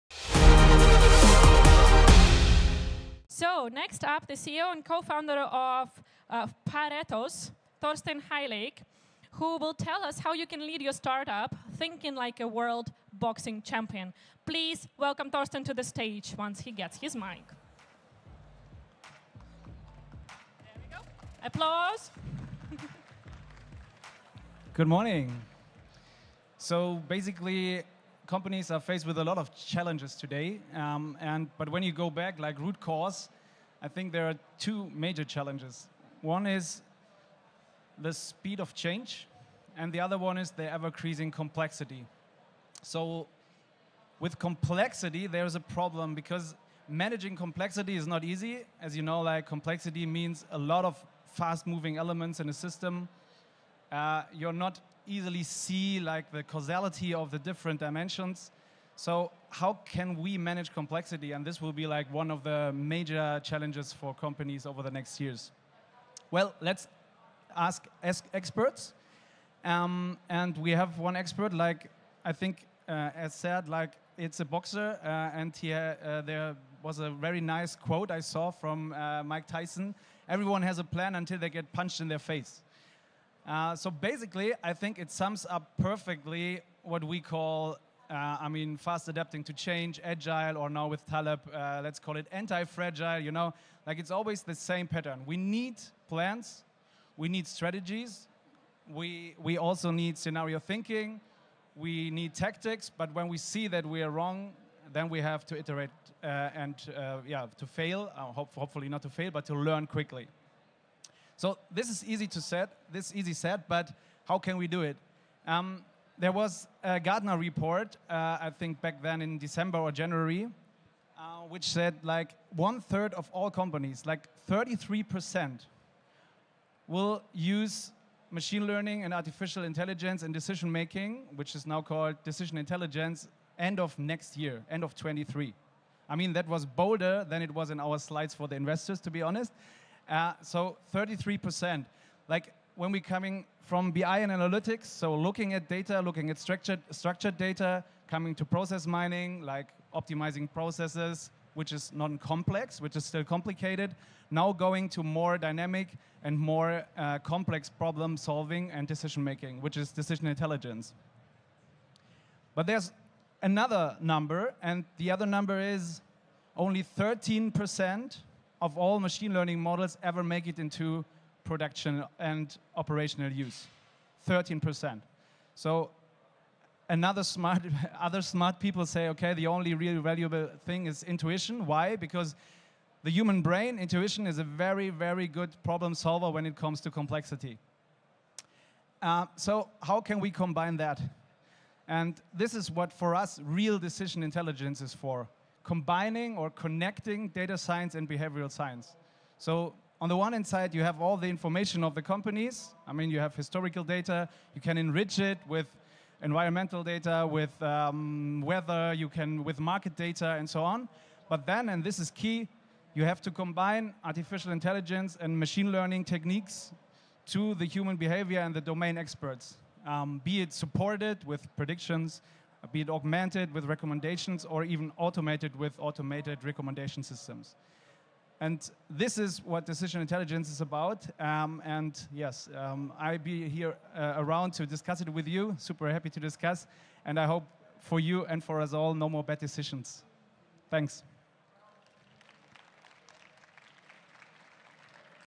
Web Summit 2022: Mike Tyson meets AI-based Decision Intelligence
Today, companies are faced with two enormous challenges: complexity coupled with an ever-increasing speed of change. Join paretos CEO & Co-Founder Thorsten Hellig as he explores how companies can overcome such challenges by thinking like a boxing world champion. Become agile, quickly adapt to punches in the face, and incorporate AI to make better business decisions.